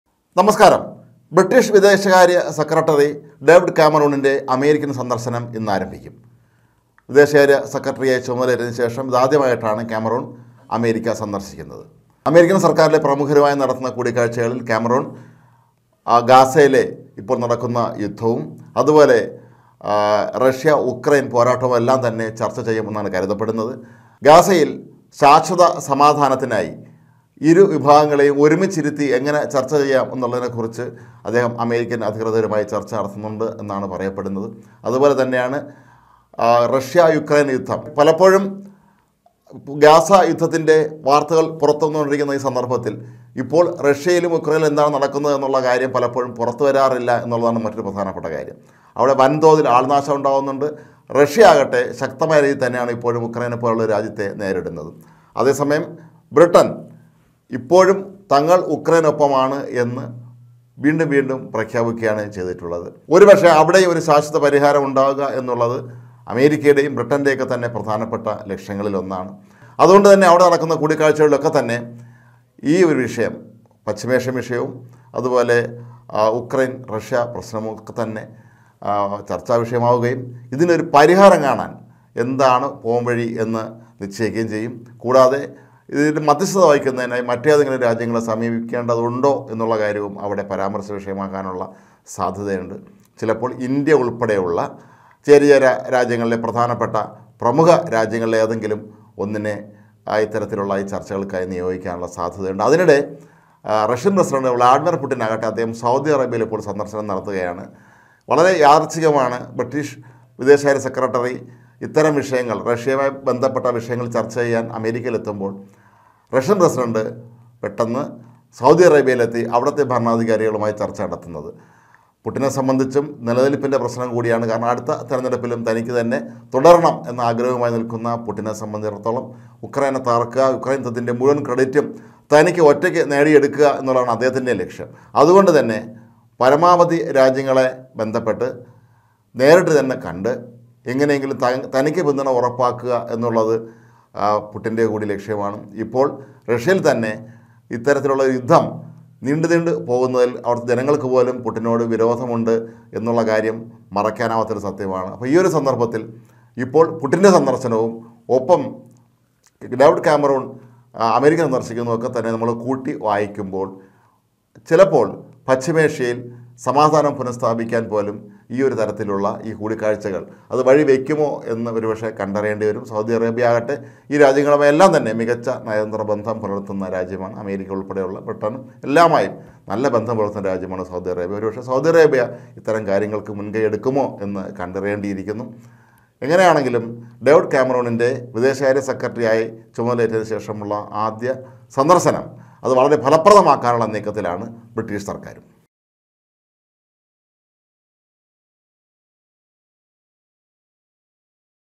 ഡേവിഡ് കാമറൂണ്‍ അമേരിക്കയില്‍ഗാസാ യുദ്ധ പരിഹാരം ചര്‍ച്ചയാകും | David Cameron to travel to US
ഡേവിഡ് കാമറൂണ്‍ അമേരിക്കയില്‍ഗാസാ യുദ്ധ പരിഹാരം ചര്‍ച്ചയാകും |   UK Foreign Secretary David Cameron to travel to US